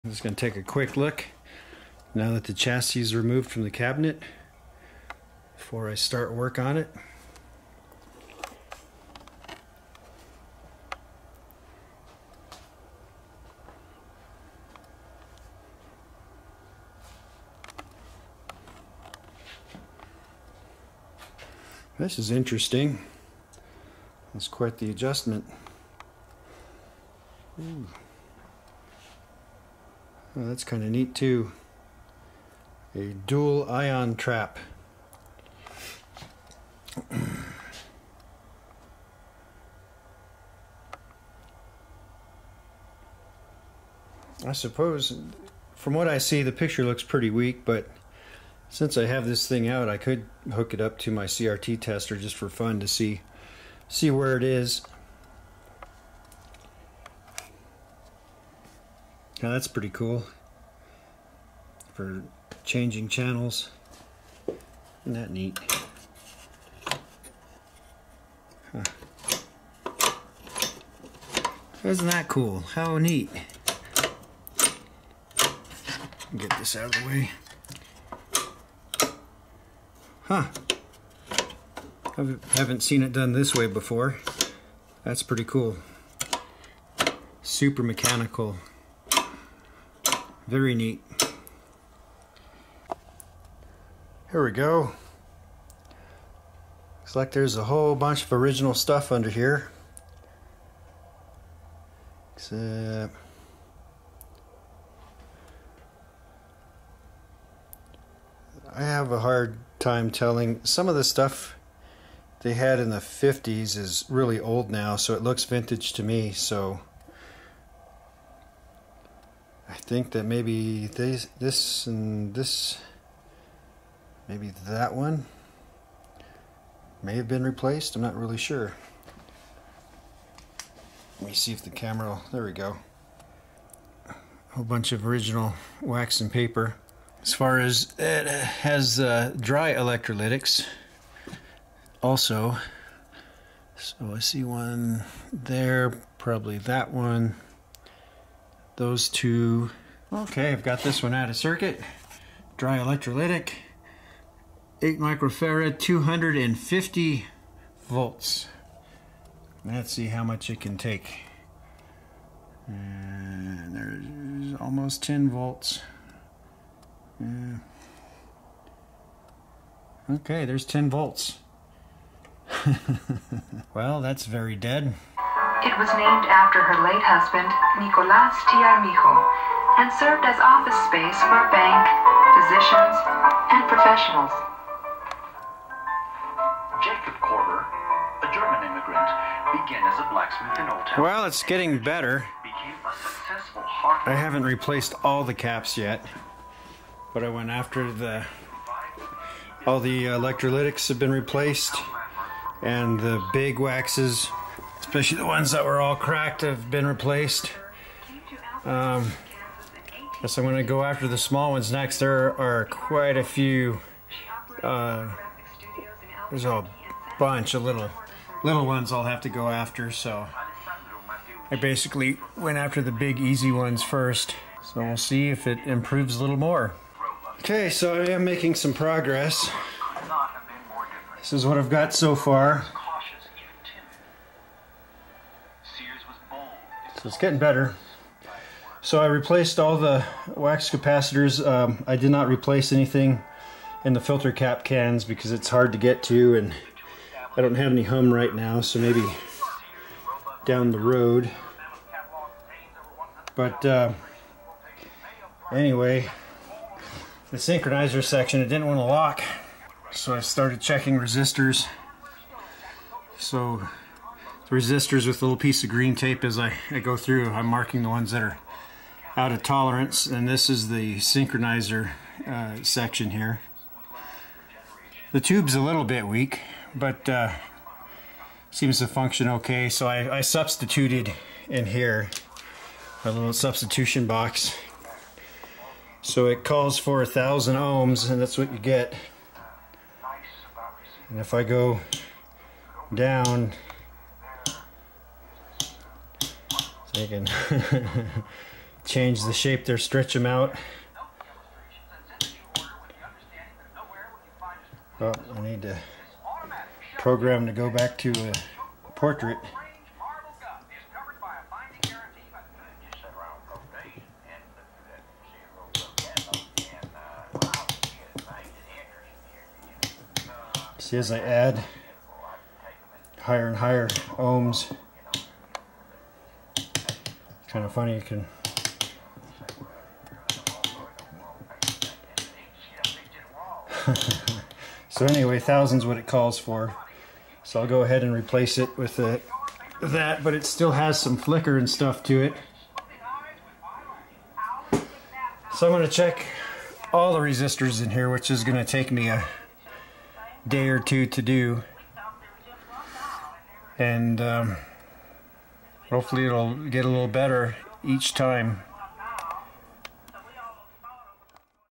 1949 Raytheon m-1105 TV Part 2
The capacitors get replaced.  Progress is made little by little.  I start on the out of tolerance resistors next.